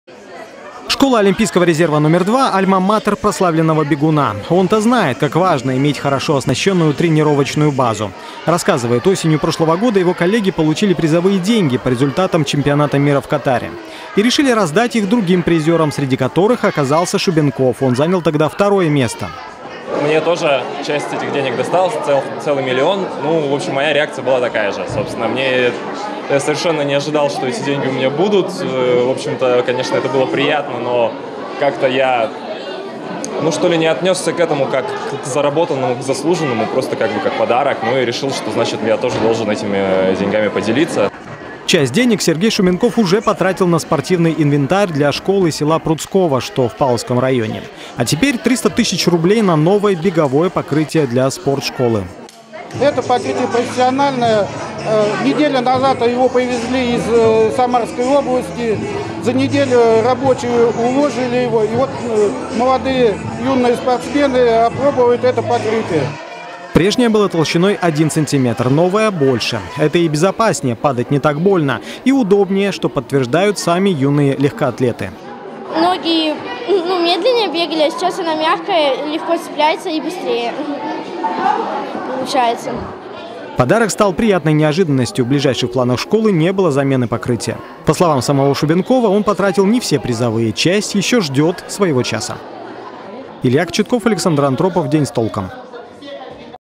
Легкоатлет Сергей Шубенков отдал призовой миллион на нужды алтайских спортшкол
Знаменитый легкоатлет Сергей Шубенков подарил родной спортивной школе новое, современное покрытие - беговые дорожки. В минувшие выходные юные спортсмены их опробовали. В чем причина неожиданного подарка и как новое покрытие поможет юным бегунам лучше заниматься?